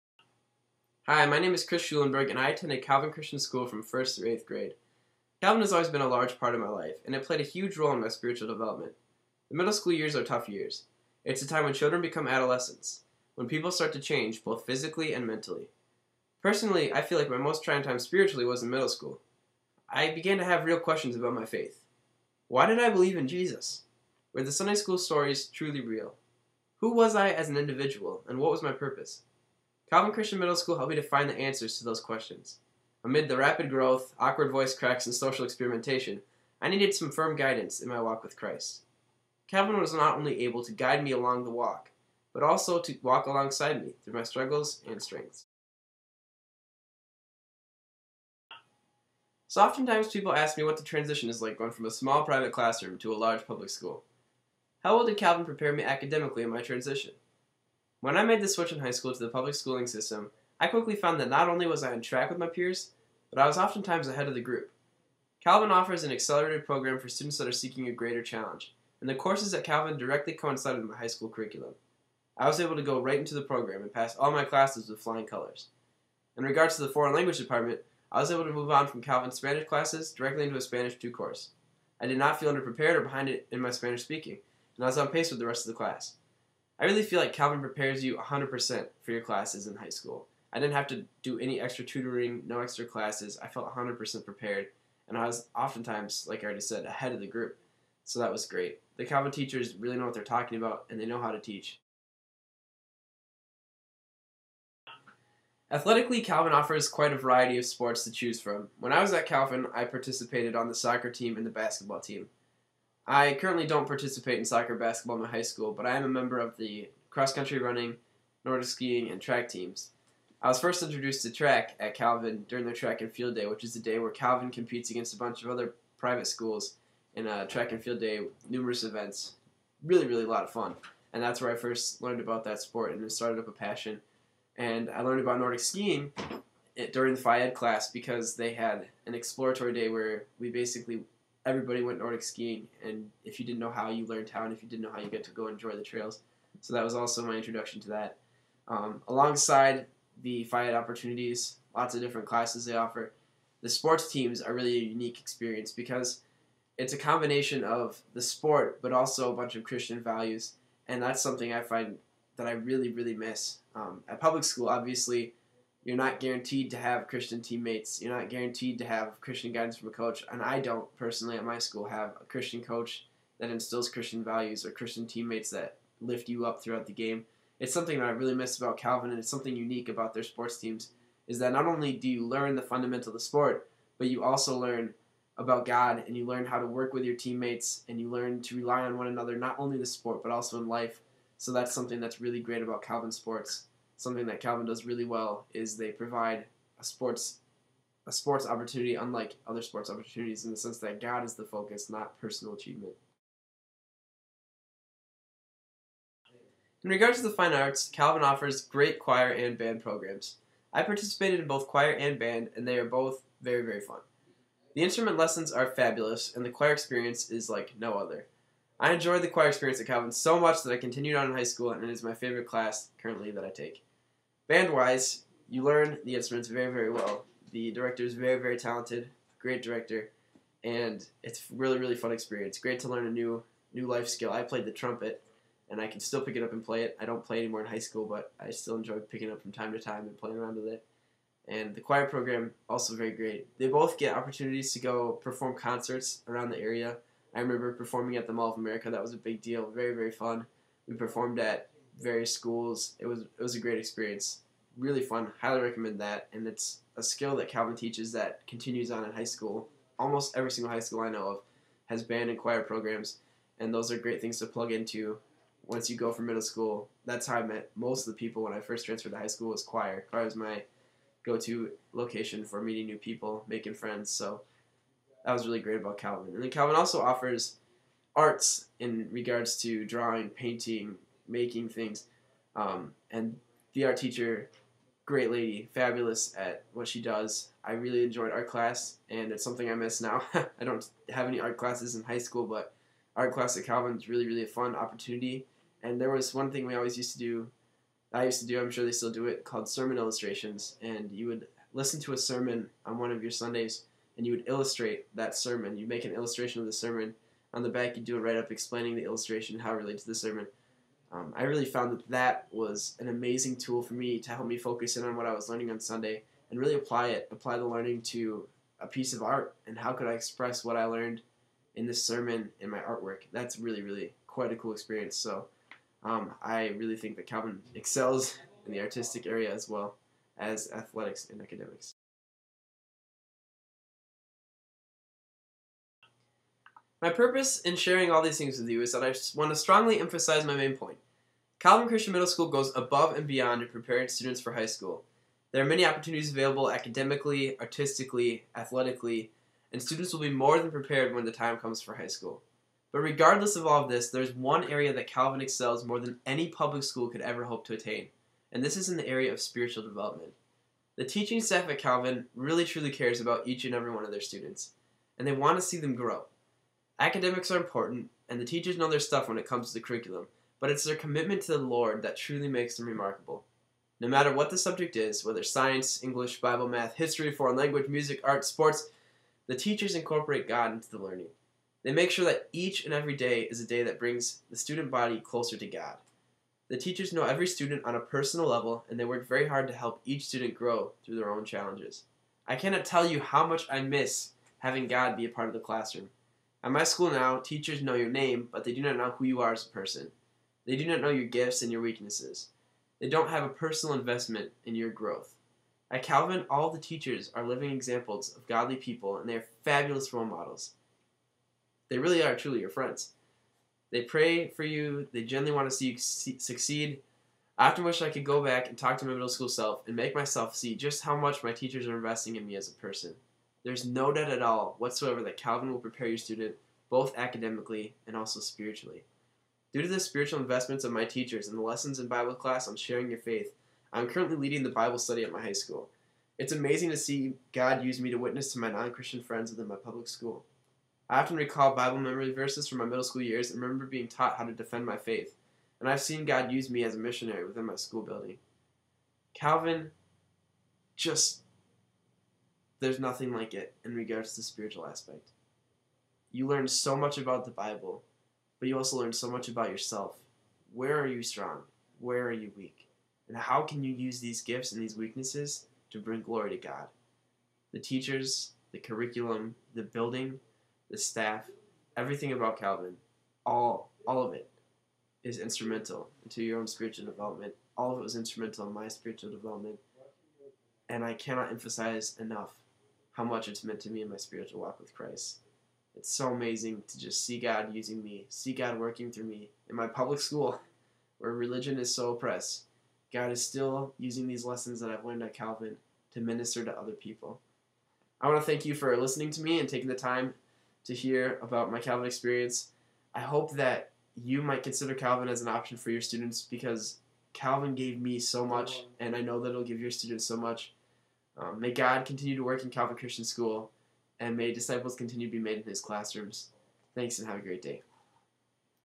Calvin Christian Middle School Student Testimony
This is a student's testimony about his experience at Calvin Christian Middle School in Edina, Minnesota.